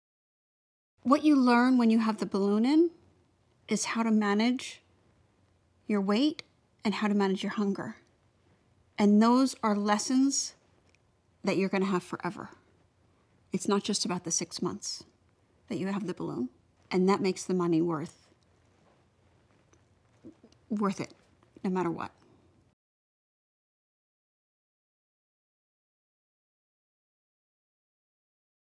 Orbera: We Help You Change Your Habits
We are proud to offer Orbera, the world's #1 intragastric balloon weight loss program that's proven to help patients experience 3.1x more weight loss than diet & exercise alone and provide the right tools and resources (such as our team of medical providers, our very own United Medical Fitness, Registered Dietitian, personal trainers and more) to successfully learn to keep it off.